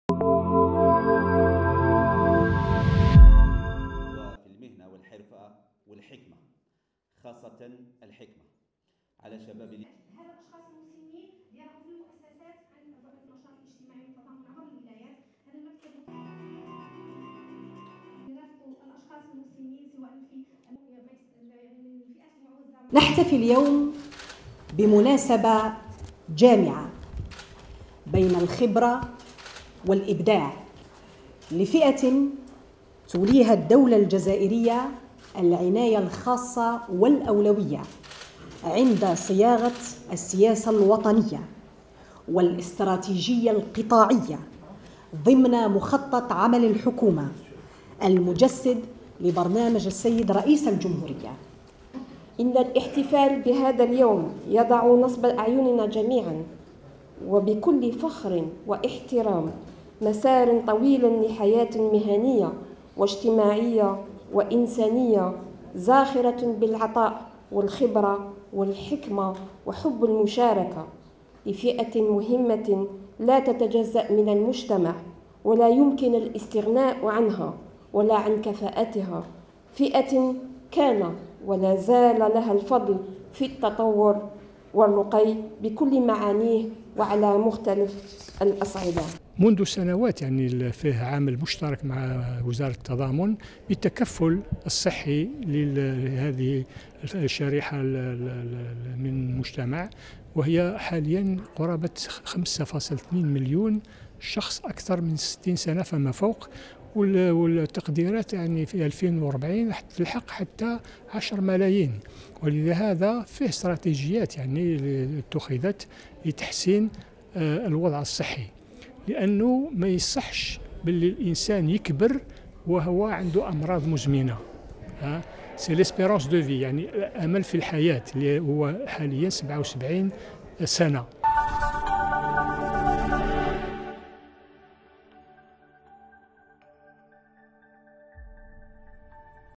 إحياء اليوم الوطني للمسن تحت شعار" الشخص المسن خبرة.. مرافقة وإبداع"
أحييت الجزائر، اليوم الوطني للمسن، لعترافا منها لهذه الفئة من المجتمع، واحتراما لها، بالنظر لمكانة المسن في العائلة الجزائرية والمجتمع الجزائري بصفة عامة، وبالنظر أيضا للمسار المهني لهذه الفئة العمرية.